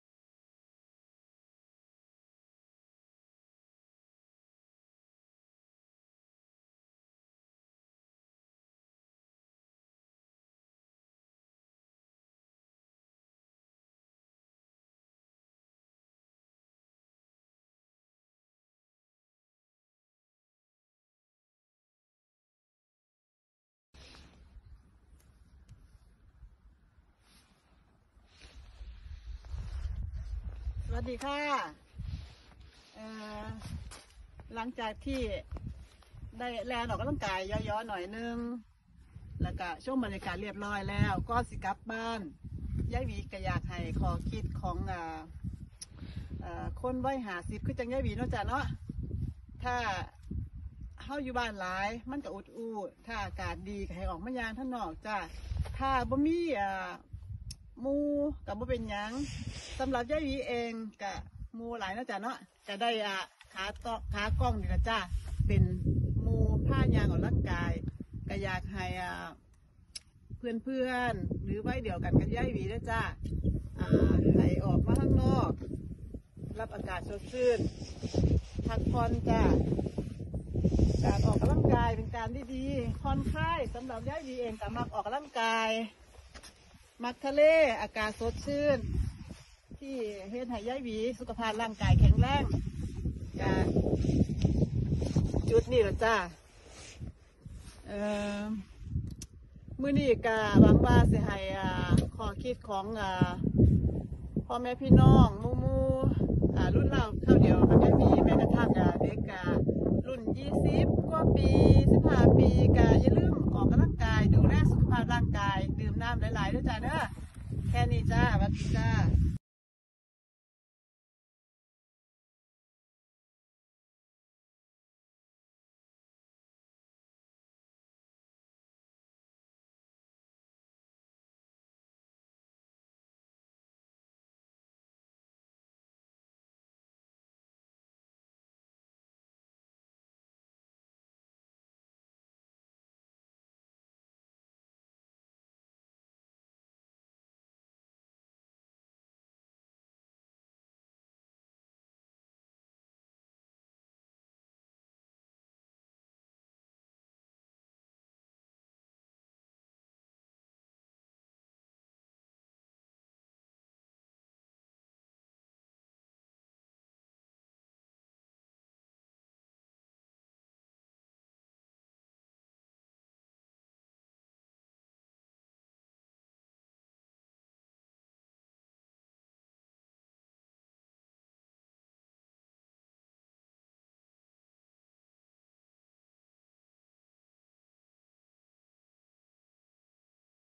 Ep90: Grosshamn ramsviklandet8/11/2020 มาดูทะเลสวีเดนกับยายจ้า เที่ยวในยุคโควิด สวีเดน ธรรมชาิบำบัด
Grosshamn ramsviklandet8/11/2020 มาดูทะเลสวีเดนกับยายจ้า เที่ยวในยุคโควิด สวีเดน

วันนี้ ยาย หวี พามาเดินออกกำลังกาย รับแสงแดด  ชม ธรรมชาติที่ทะเลใกล้บ้านจ้า

Grosshamns fältstation.
 Grosshamns Fältstation ligger på Ramsvikslandet. Grosshamn som en gång fått sitt namn efter familjen Gross.

สถานที่ชื่อว่า Grosshamn ramsviklandet

เมือง Hunnebostrand Sweden

ขึ้นกับอำเภอ : Sotenäs kommun

พื้นที่แห่งนี้กว้างมากจ้า มีทางเข้า ออกหลายทาง เป็นที่นิยมของนักท่องเที่ยวและผู้คนที่อาศัยอยู่เมืองใกล้ๆ เช่น Kungshamn ,Bovallstrand, Hunnebostrand , hovenäset , และบริเวณใกล้เคียงส่วนใหญ่มาเดินออกกำลังกาย ปิคนิค ตกปลา ปั่นจักรยาน

เป็นสถานีค่ายทหารในยุคสงครามโลก แต่เดียววันหลังจะพาไปดูจ้า

ฝากเพื่อนๆเป็นกำลังแรงใจ ให้ ยายหวี